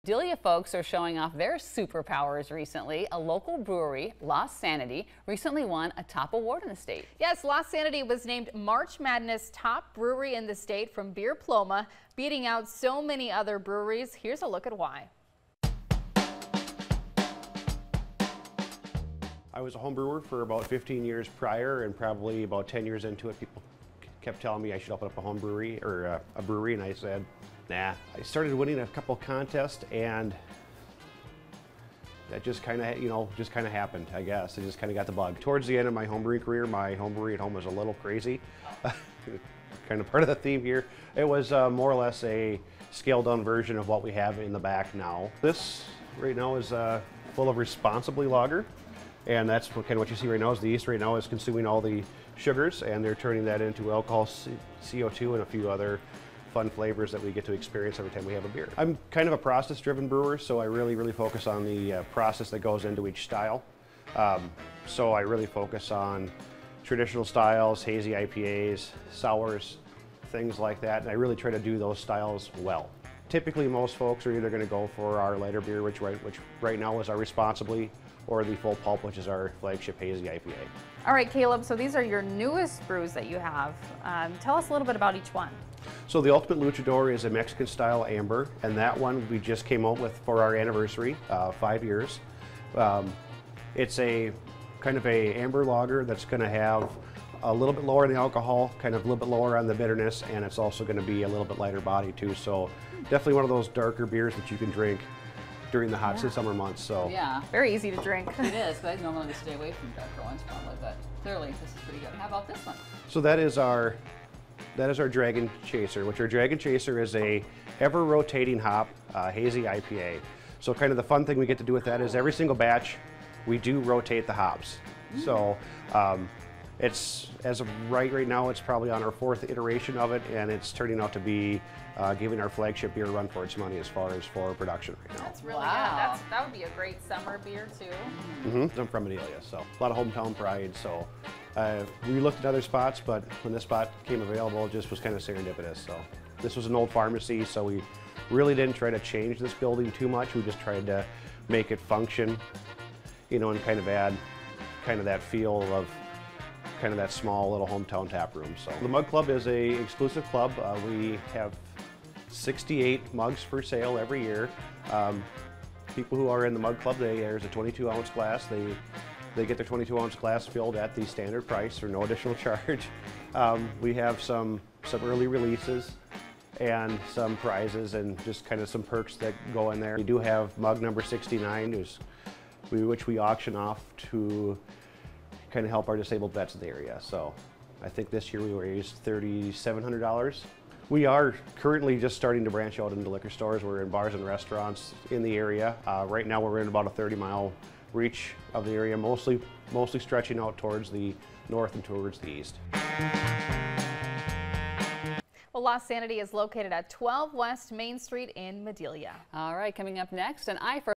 A method to March Madness: Lost Sanity brewery in Madelia
Some Madelia folks showing are off their super powers recently. A local brewery, Lost Sanity, recently won a top award in the state!